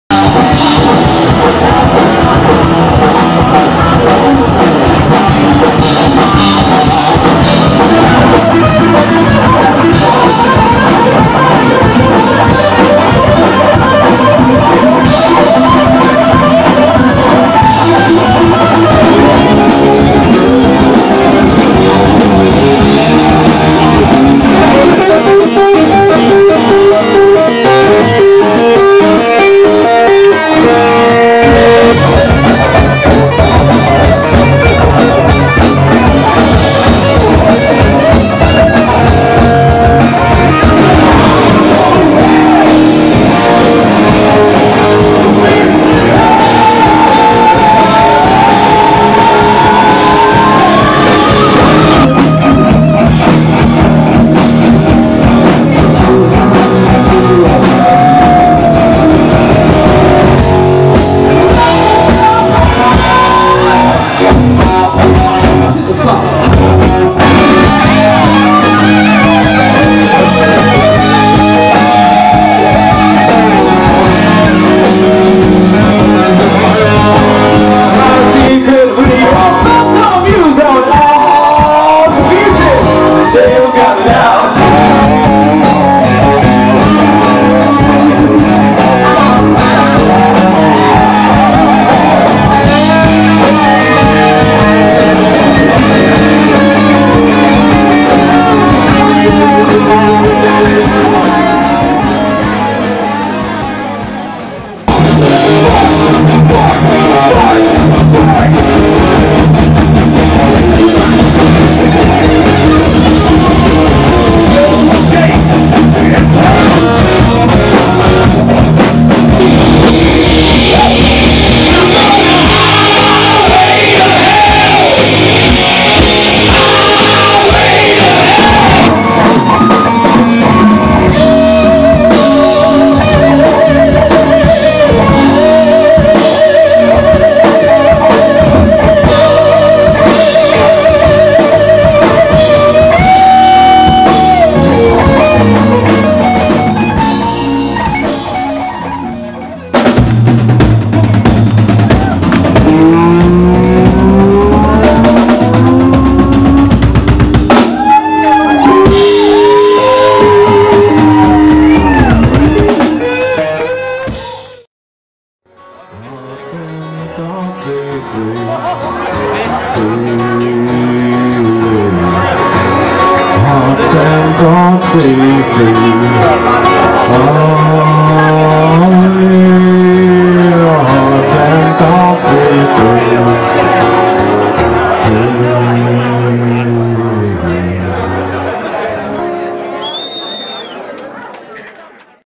CCL Video - Jamming for Joe Celebration
Jamming for Joe Celebration. Hanover, Pa (June 27, 2015)
Problem Child, Half Serious, Back Street Kids, Steel, members of Big Jack, Chain Links, Thane Farace, John Christ. Army of Dogs, Lauren Hill,